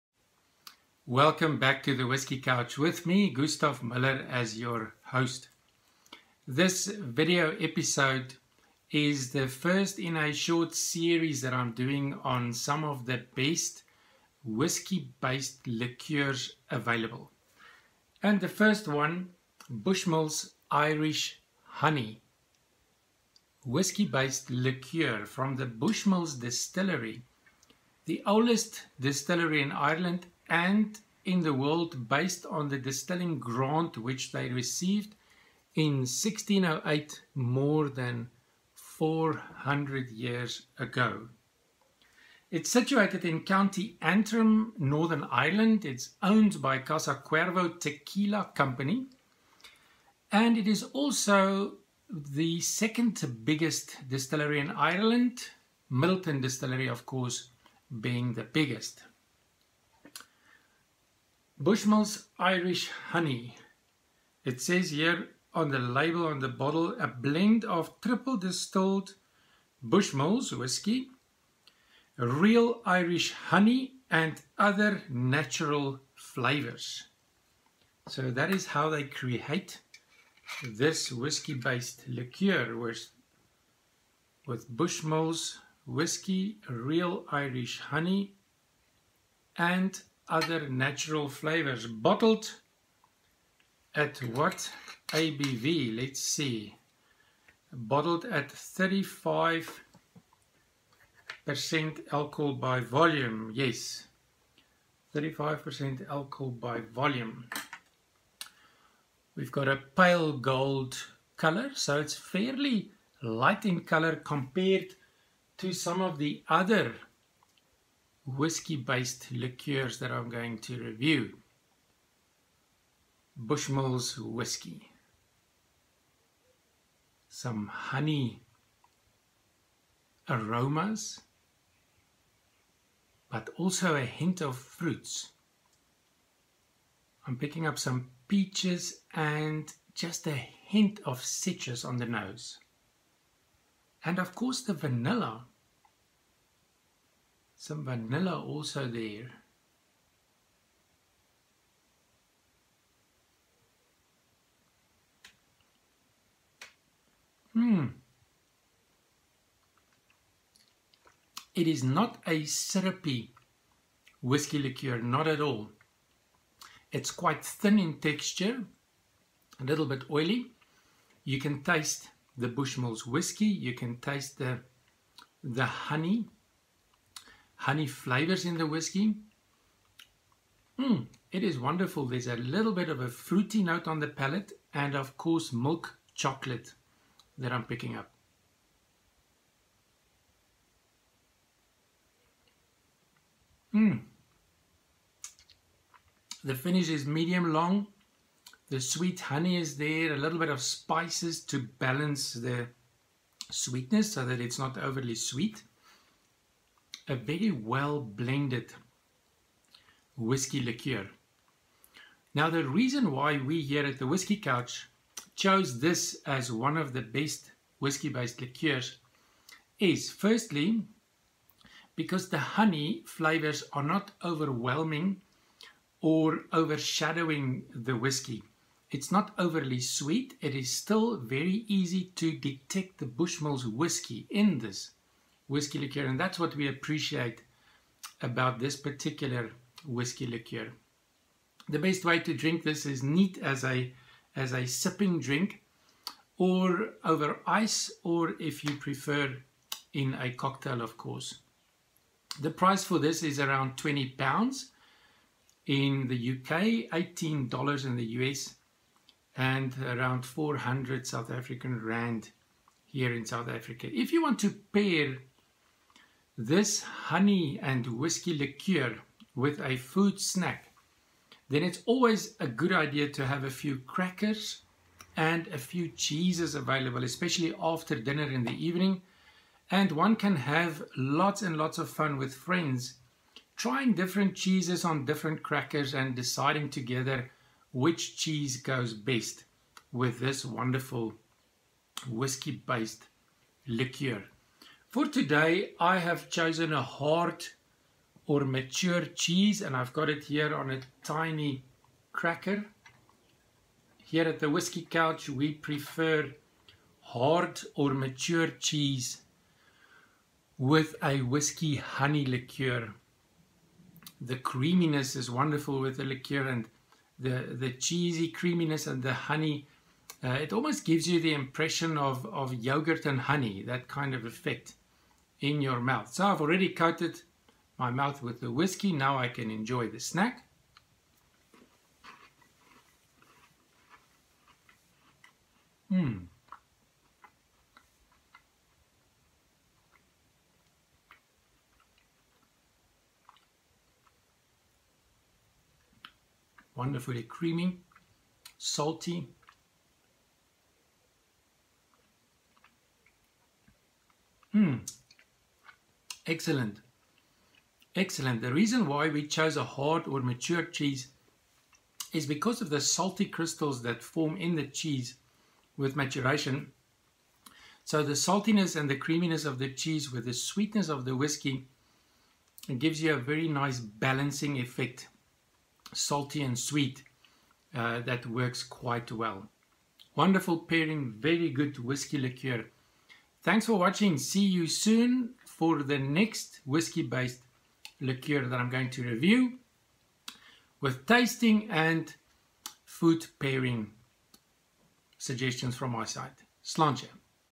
Best Whisky Liqueurs - Bushmills Irish Honey: Tasting & Food Pairing, Review #58
BUSHMILLS IRISH HONEY

DISTILLERY
Situated in County Antrim, Northern Ireland, owned by Casa Cuervo (Tequila company)
The oldest distillery in Ireland and the world based on the distilling grant they received in 1608
Second biggest Irish distiller after Middleton
THIS WHISKY LIQUEUR
Blend of tripple distilled Bushmills whiskey, real Irish Honey and other natural flavours
ABV 35%

COLOUR
Pale Gold
NOSE
Bushmills whisky, honey, fruit (peaches and citrus), vanilla
TASTE
Not syrupy, whisky, honey, fruit, milk chocolate
FINISH
Medium long, sweet honey, spices, balanced, very well blended
ENJOY: neat, over ice or in a cocktail
COST: … $18 … £20 … R400

FOOD PAIRING RECOMMENDATIONS
Crackers with a selection of cheese, mature or hard cheese works best with honey flavoured whisky liqueur (it creates a yougurt and honey effect on the palate), nutty shortcake also works well

To comply with Google Adsense policy this video is an independent and unbiased whisky review based on personal opinion only. It is not an advertisement to sell the product.